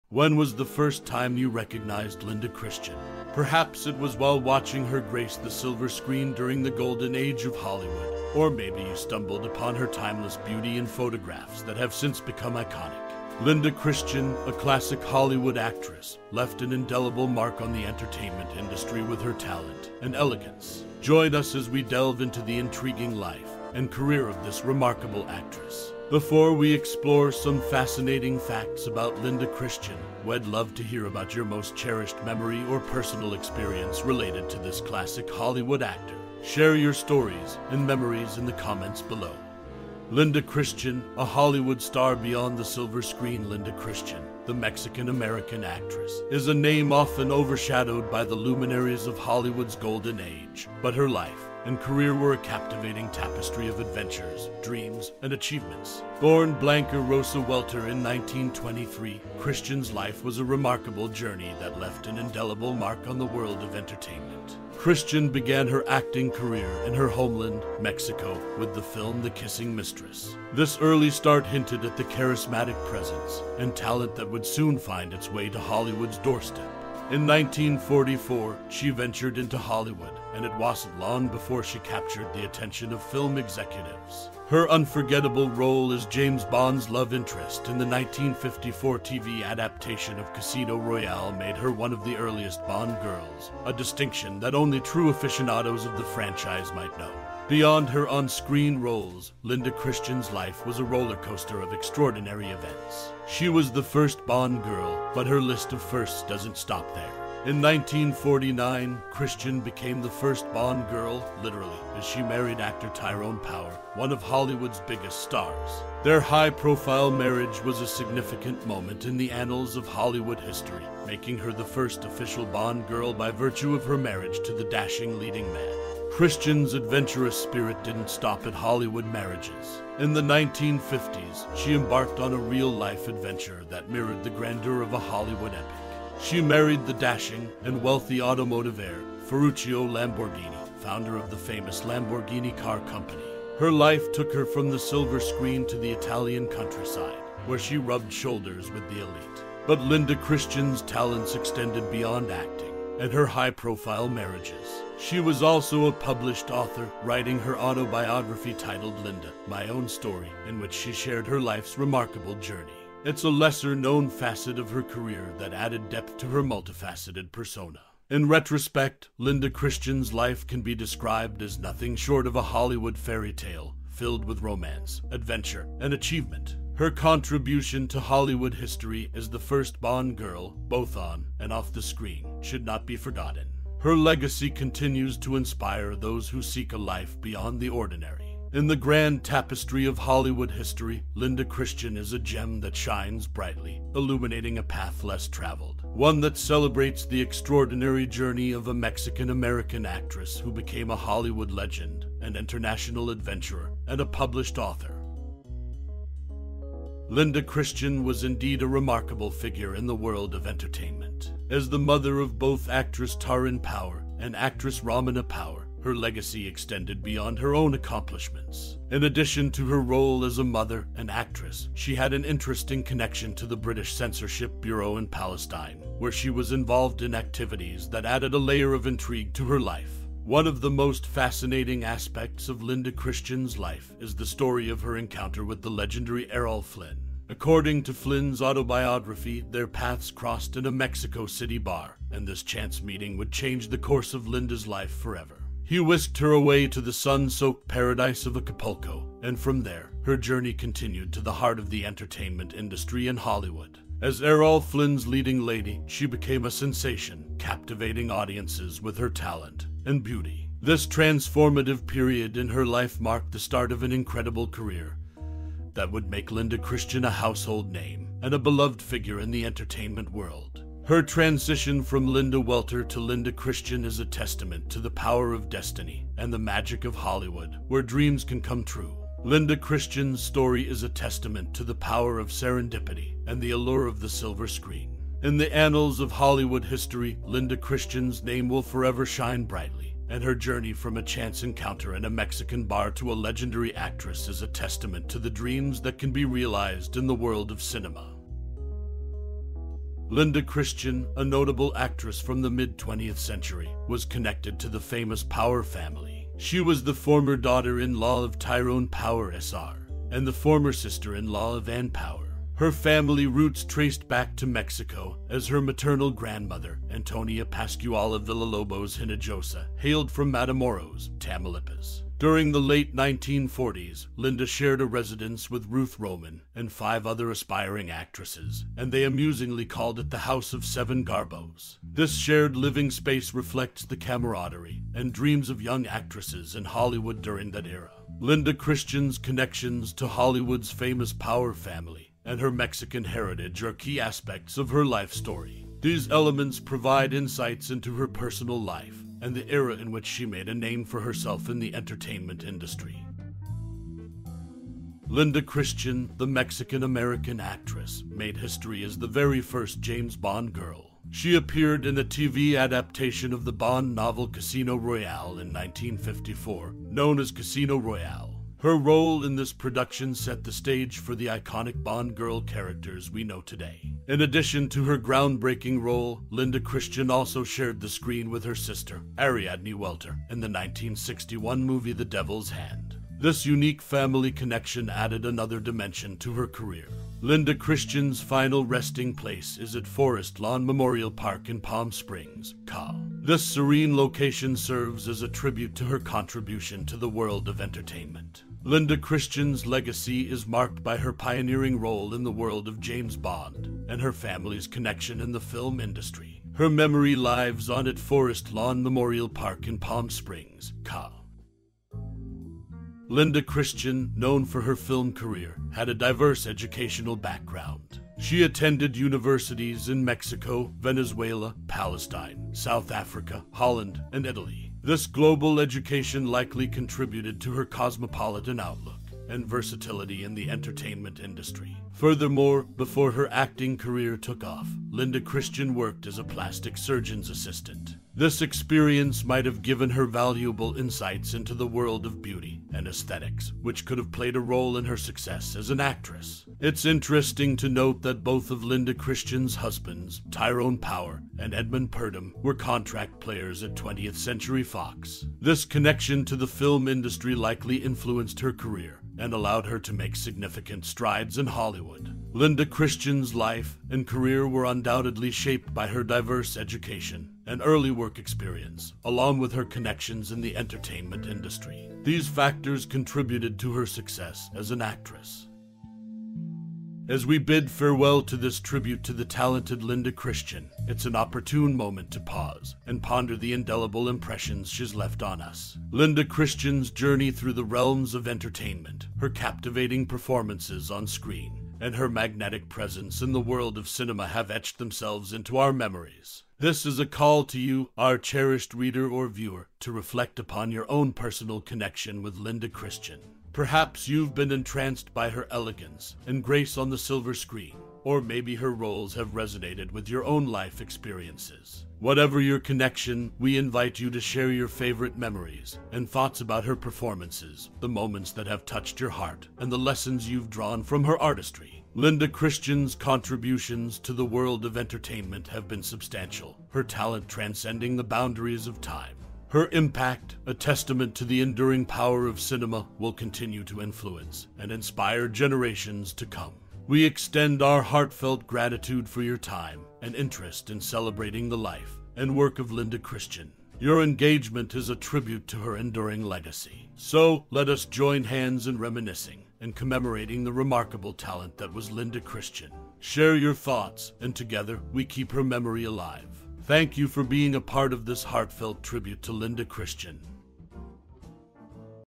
TikTok Feedback Suggests: Linda Christian Is an Unacknowledged Figure for the Youth
Step back in time with me as we pay tribute to one of Classic Hollywood's brightest stars - Linda Christian. 🌟 From captivating performances to timeless elegance, Linda Christian's influence on the silver screen remains unrivaled.

🎥 Join me as we uncover the stories behind Linda Christian's most iconic roles and the moments that shaped their legendary journey. 🌠

🎬 Lights, camera, and let's immerse ourselves in the magic of Classic Hollywood with Linda Christian leading the way. ✨🎭
Filmography:
Cambiamento d'aria
Amore inquieto di Maria
Delitti
For All the Gold in the World
The World's Gold
How to Seduce a Playboy
Murder in Amsterdam
The Boy and the Ball and the Hole in the Wall
The Moment of Truth
Contest Girl
Full Hearts and Empty Pockets
The V.I.P.s
The Dick Powell Theatre
The Lloyd Bridges Show
The Alfred Hitchcock Hour
Passport for a Corpse
The Devil's Hand
Das große Wunschkonzert
Appuntamento a Ischia
Meet Peter Voss
Rebel Flight to Cuba
The House of the Seven Hawks
ITV Television Playhouse
Thunderstorm
Athena
Climax!
Slaves of Babylon
The Happy Time
Battle Zone
Show Boat
Tarzan and the Mermaids
Green Dolphin Street
Holiday in Mexico
Club Havana
Up in Arms
The Rock of Souls